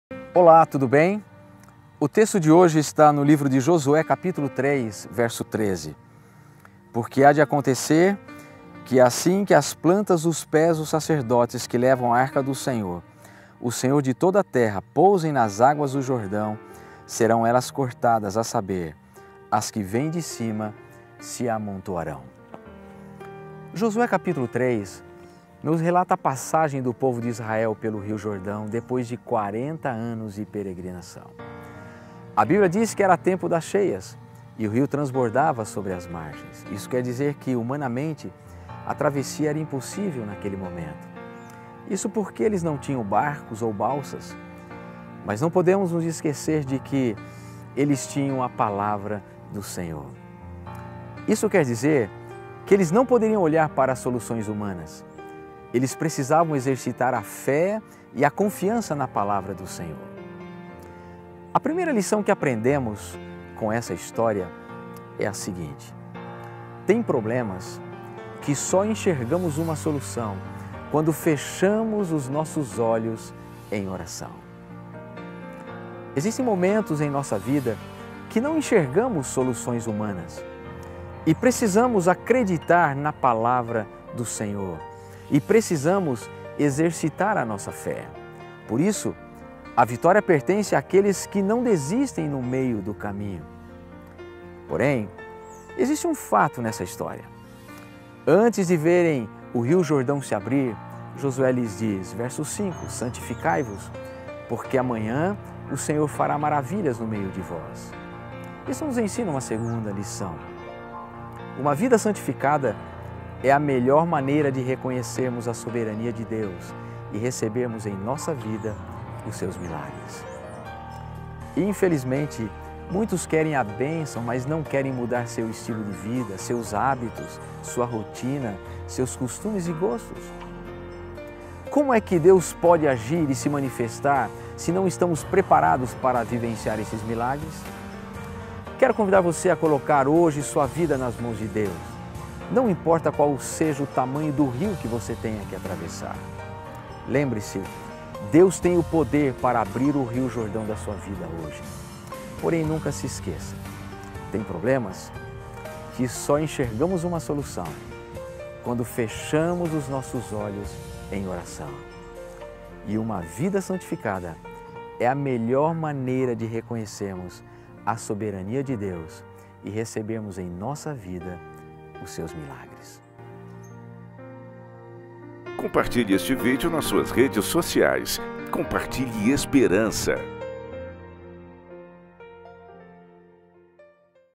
O Deus do impossível e a resposta de fé do crente diante das lutas e provações da vida
Josué, capítulo 3, nos relata a passagem do povo de Israel pelo rio Jordão, depois de quarenta anos de peregrinação. A Bíblia diz que era tempo das cheias e o rio transbordava sobre as margens... — Pr. Elias Brenha (transcrito a partir de conteúdo audiovisual licenciado pela Igreja Unasp EC). Thumbnail: foto de Wirestock / Freepik.